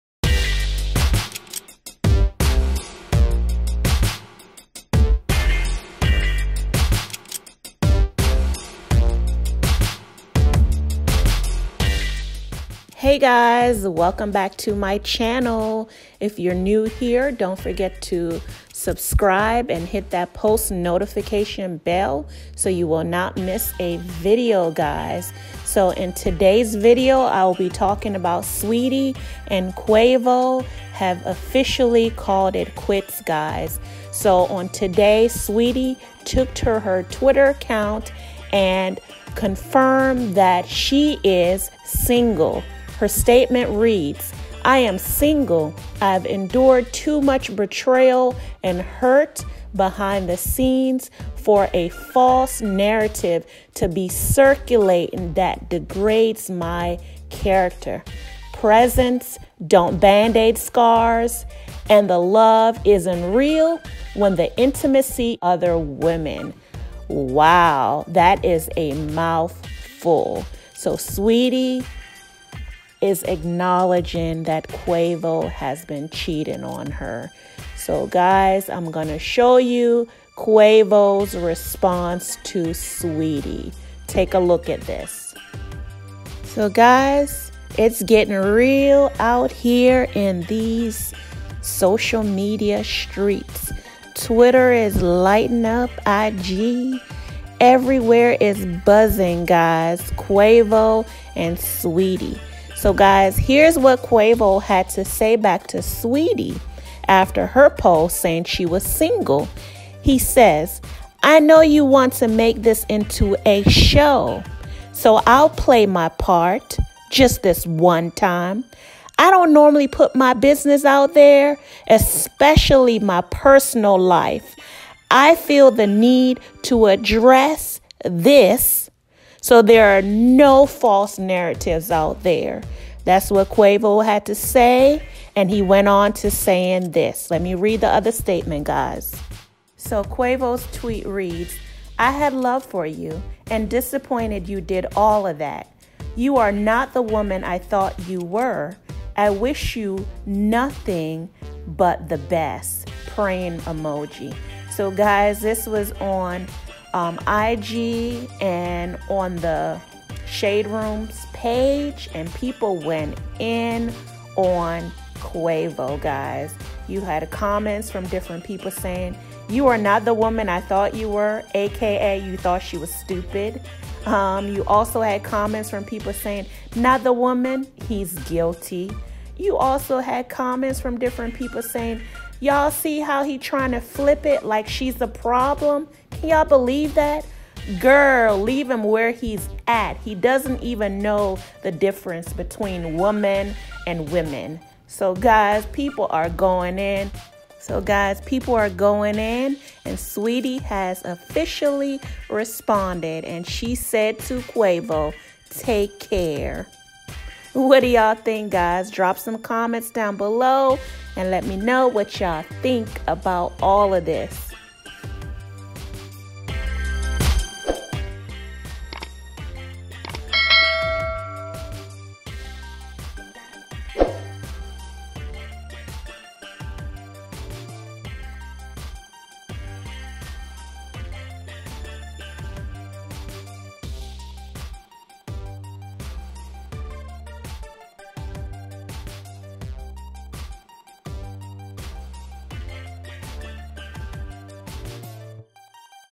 Quavo and Saweetie messy Break up Are They Petty ?
Although, Sweetie Dumps Quavo because He cheated on Her. Will Sweetie Want Quavo Back ? Quavo Takes Back Bentley He Gifted Her. Drop some comments and let’s talk 🙌🏽❤️

Shelly